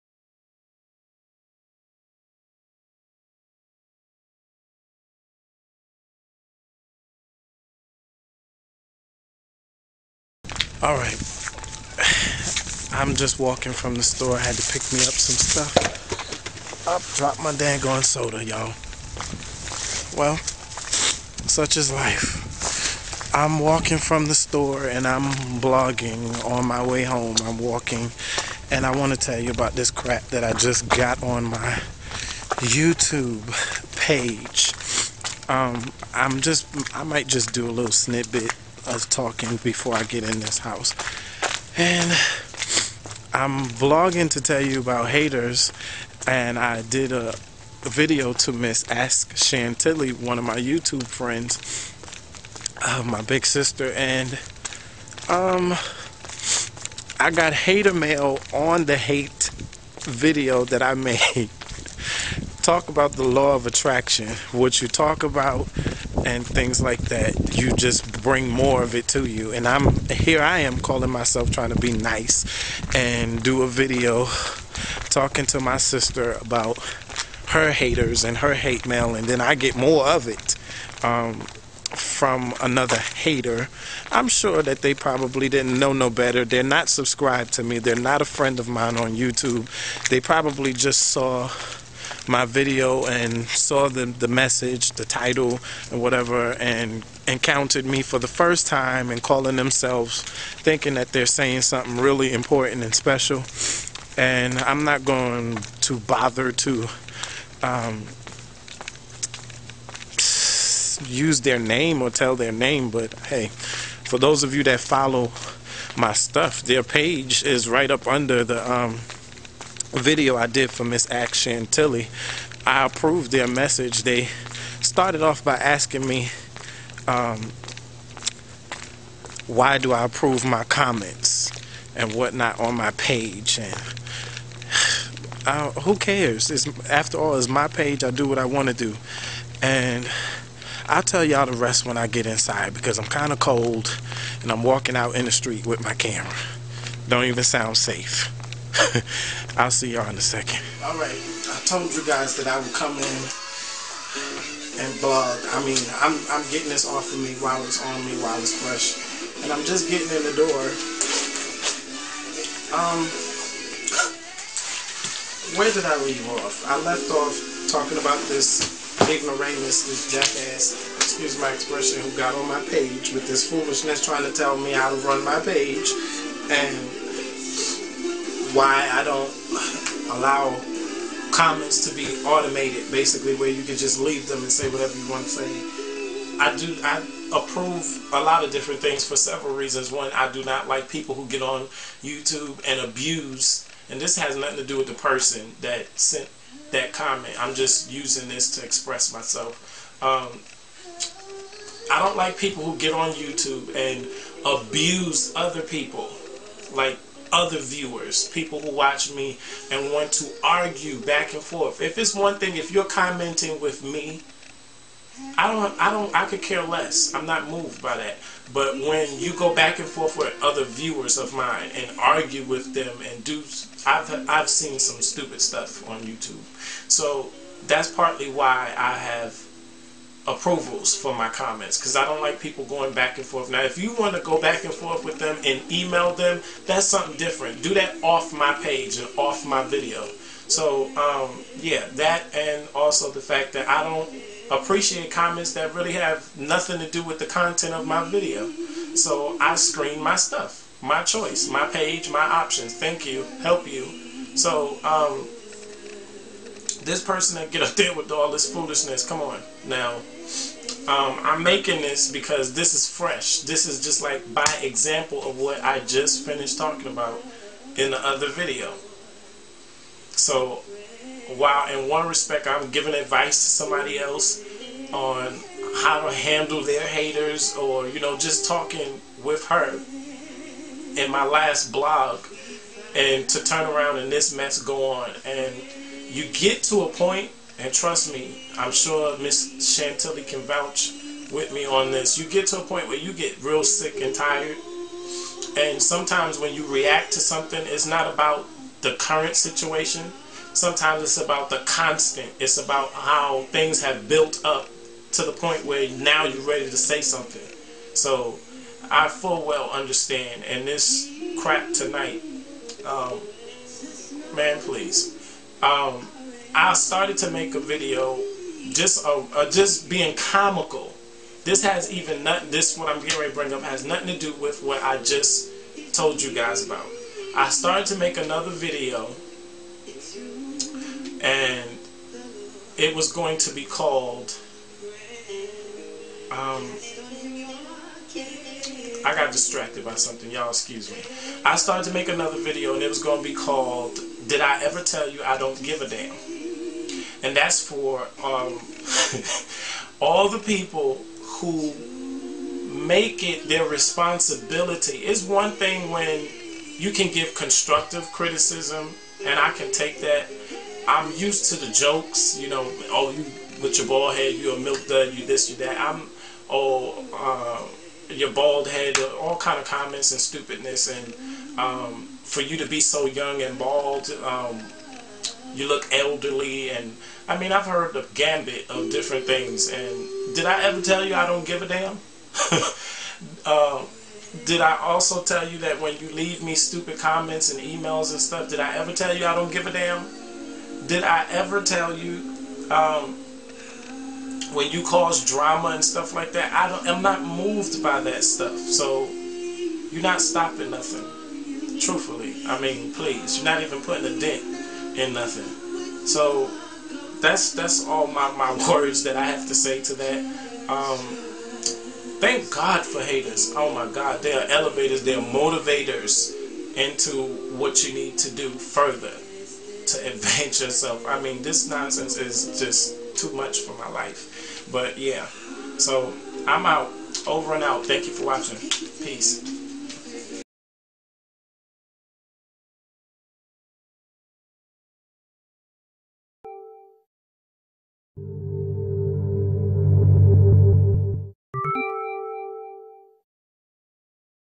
Hate Mail:Michael London After Dark :Season 2: Vlog#4
Hate Mail: Michael London talks about a random viewers comment sent to him. - for Clothing!